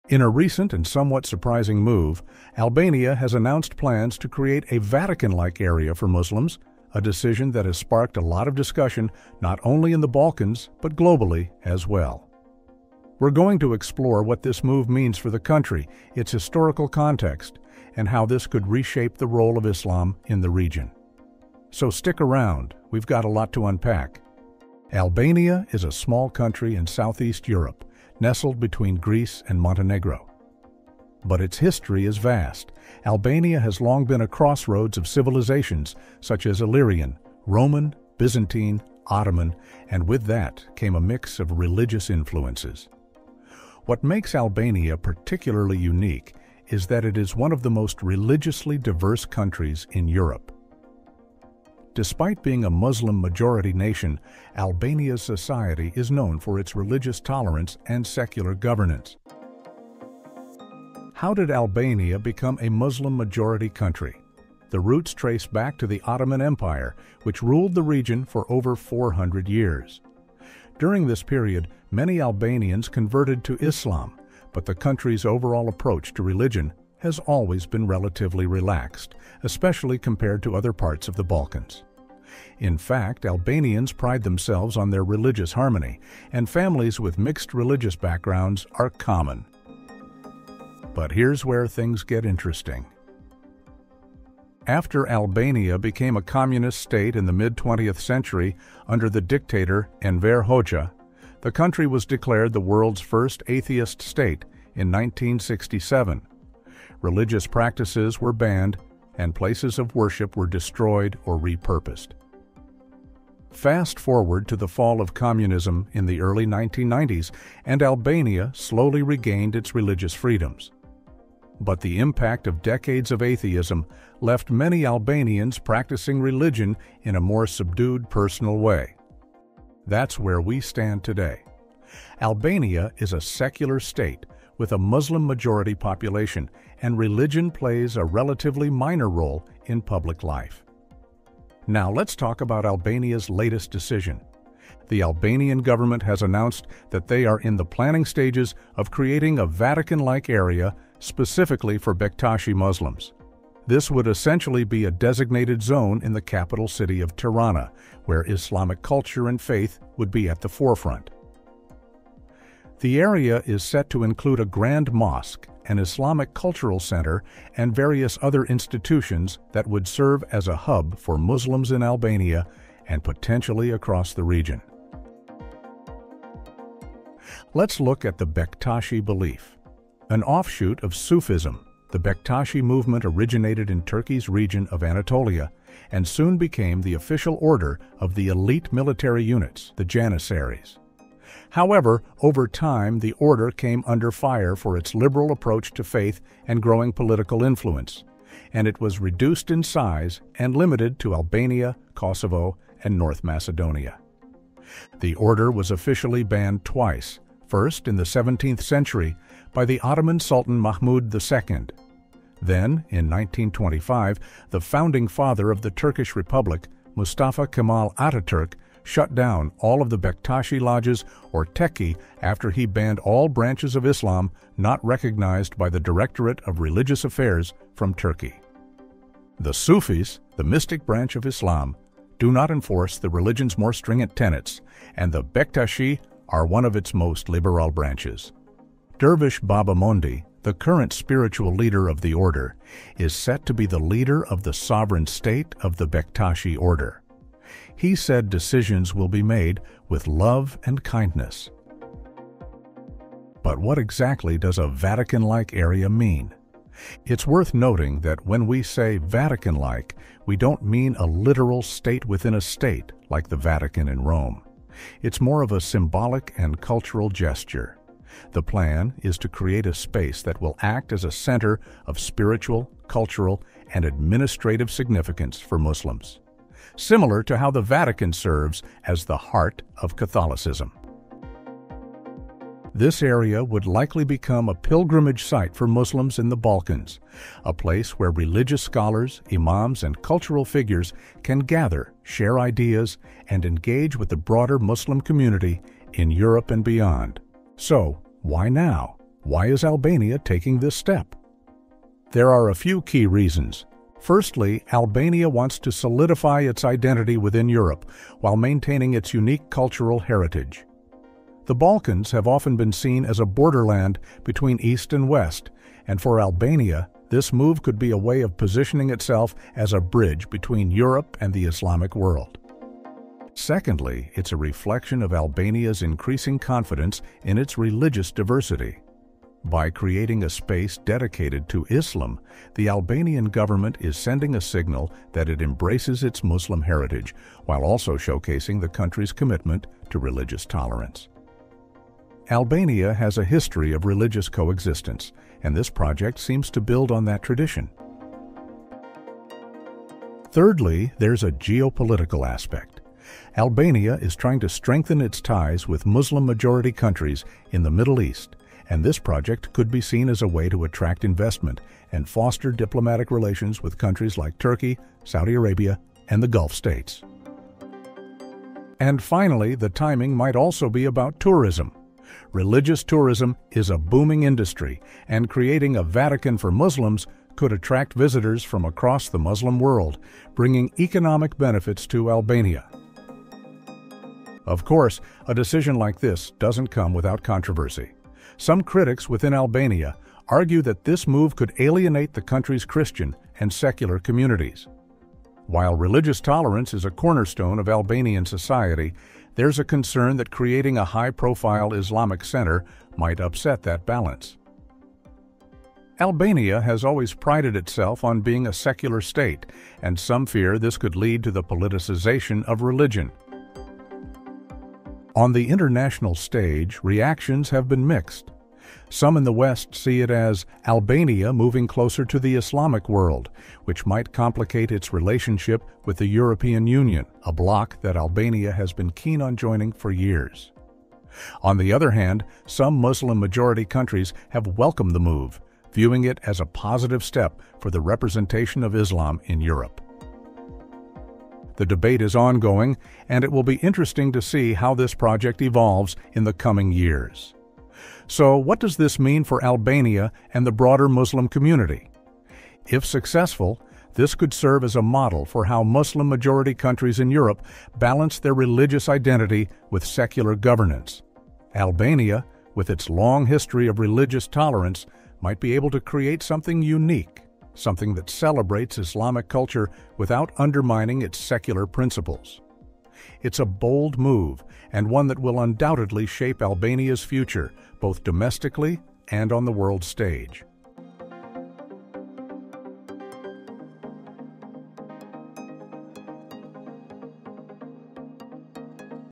Albania will establish Muslim Vatican in Europe
In a recent and somewhat surprising move, Albania has announced plans to create a Vatican-like area for Muslims. This decision has sparked a lot of discussion not only in the Balkans but globally as well. We’re going to explore what this move means for the country, its historical context and how this could reshape the role of Islam in the region. So stick around, we’ve got a lot to unpack.